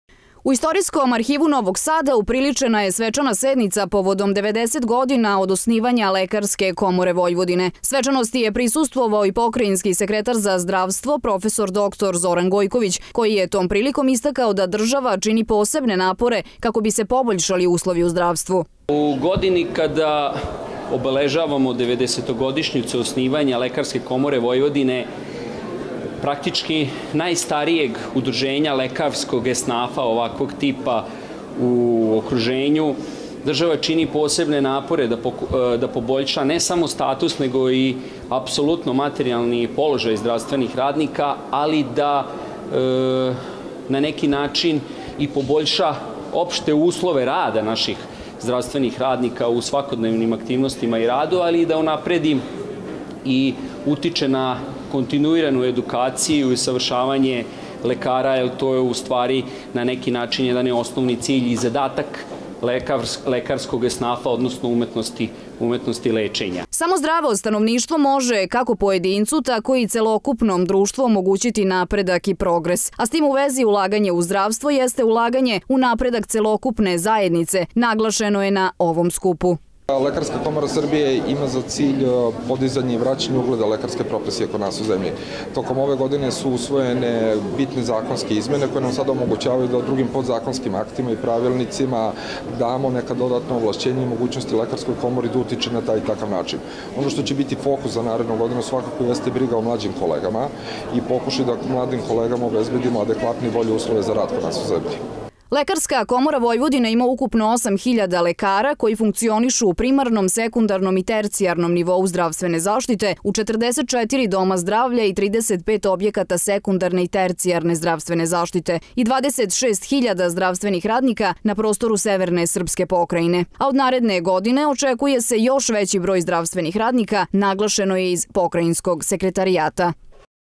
Lekari u Vojvodini obeležili jubilej
Svečanom sednicom Skupštine Regionalne lekarske komore Vojvodine obeleženo je 90 godina od osnivanja Lekarske komore Dunavske banovine i 15 godina od ponovnog početka rada Lekarske komore Vojvodine.

Godine 1929. Ministarstvo socijalne politike i narodnog zdravlja Kraljevine Jugoslavije donelo je odluku o novoj teritorijalnoj podeli lekarskih komora u državi. Na tadašnjoj teritoriji koju je pokrivala Lekarska komora za Srbiju, Vojvodinu i Srem postojale su četiri lekarske komore sa ukupno 2000 članova. Danas samo Regionalna lekarska komora Vojvodine ima preko 7800 članova.

Obraćajući se prisutnim članovima Skupštine, pokrajinski sekretar za zdravstvo prof. dr Zoran Gojković podsetio je na to da nema većeg napretka za jedno društvo od zdrave nacije. Zato država, kako je rekao sekretar Gojković, kontinuirano radi na poboljšanju materijalnog statusa i uslova rada svih zaposlenih u zdravstvenom sektoru, ali i na edukaciji i usavršavanju lekara, jer je to osnova za zdravu populaciju, a samim tim i za prosperitet društva u celini.

Lekarska komora Vojvodine osnovana je 2004. godine kao nezavisna i profesionalna organizacija lekara, a od 2005. godine, kada je donet Zakon o komorama, nastavila je sa radom u okviru Lekarske komore Srbije, kao Regionalna lekarska komora Vojvodine.